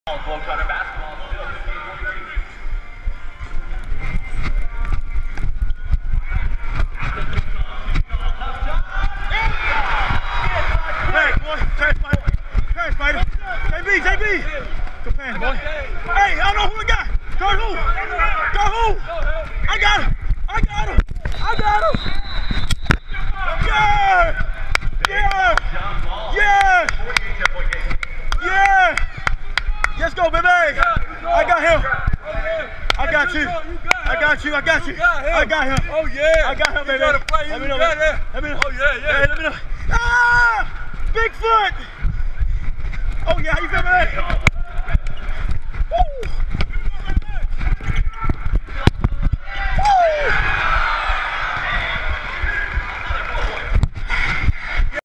Harlem Globetrotters Game: Player Cam
Too Tall wore a Go Pro camera during a game so fans could see EXACTLY what it's like to be in the action! Clips like this and other exclusive content is available to members of the Globetrotters fan club. Sign-up today